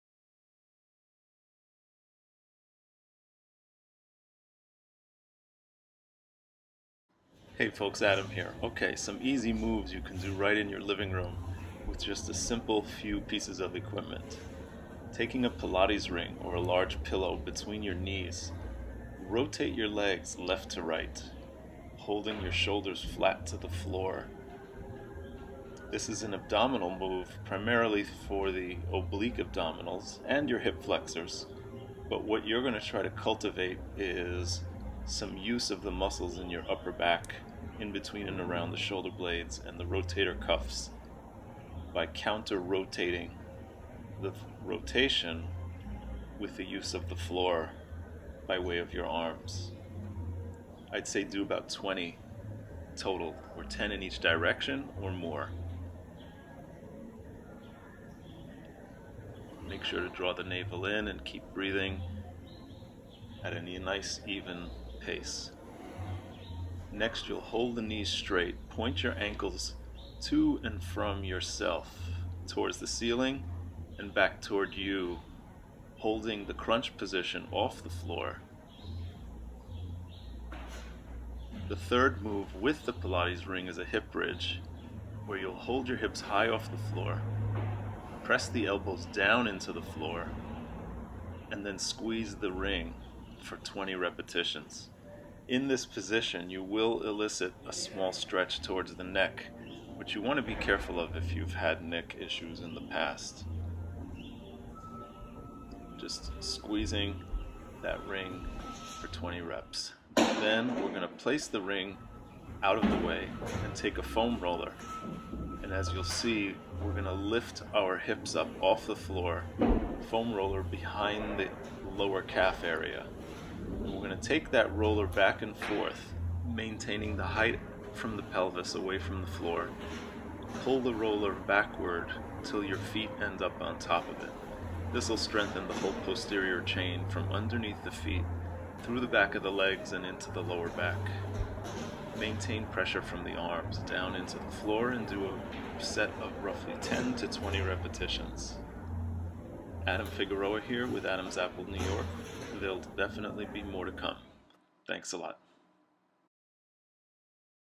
4 moves for the abs and lower back
Here are 4 moves to train the abdominals and lower back at home. If any of these are uncomfortable to execute, skip over it. Aim for 10 to 30 repetitions and do 3 rounds in a row.